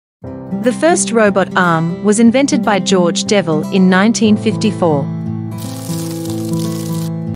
The First Robot Arm: George Devol's Game-Changing Invention | SGK English
Discover the groundbreaking invention that transformed the manufacturing industry forever. In 1954, George Devol introduced the world to the first robot arm, revolutionizing automation and paving the way for a new era of technological advancements. Dive into the fascinating history behind this pivotal moment, exploring the impact of Devol's invention on industries such as automotive, electronics, and beyond. Learn about the evolution of robot arms since then, from early prototypes to the sophisticated robotic systems of today. Join us on a journey through time and witness the birth of a technological marvel that has shaped the world we live in.